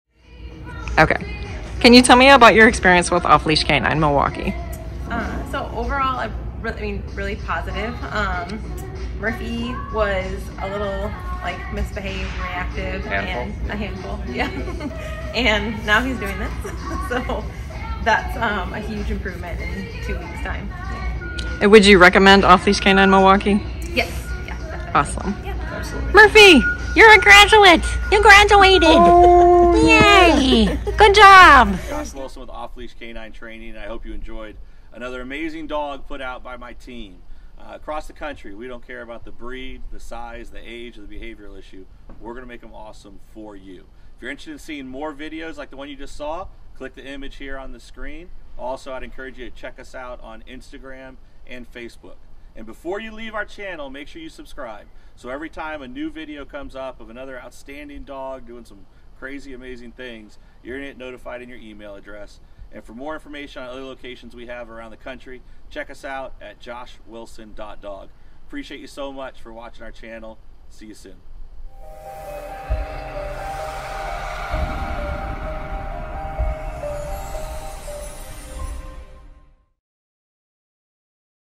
2 Y/O Aussiedoodle "Murphy"/Owner Testimonial /Ecollar Training
Listen to the heartwarming story of Murphy, a 2-year-old Aussiedoodle, as shared by his proud owner in this inspiring testimonial. Discover how E-Collar training has transformed Murphy's behavior and strengthened the bond between him and his devoted owner.